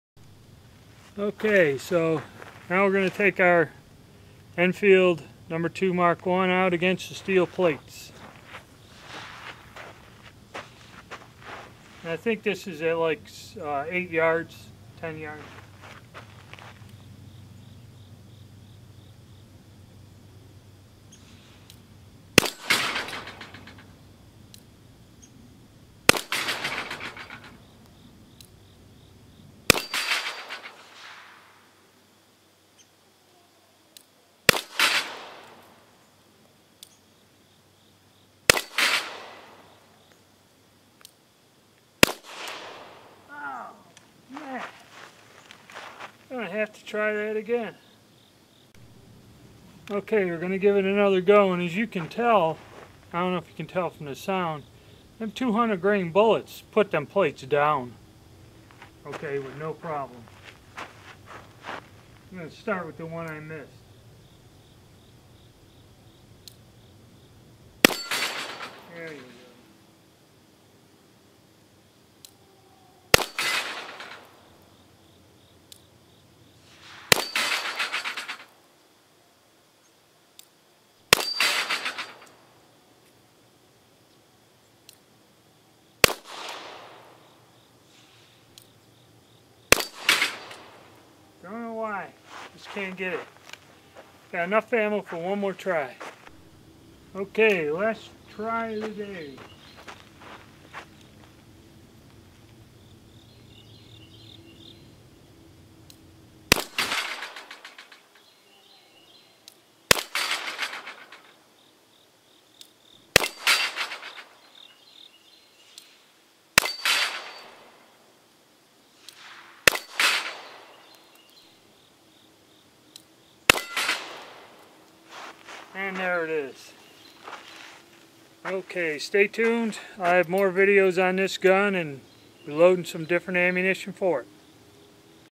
Shooting at steel plates w/ the Enfield No. 2 MK1* Pistol
I give the Enfield a try at the steel plates, surprisingly that heavy bullet going at 600 fps knocks the plates down with some force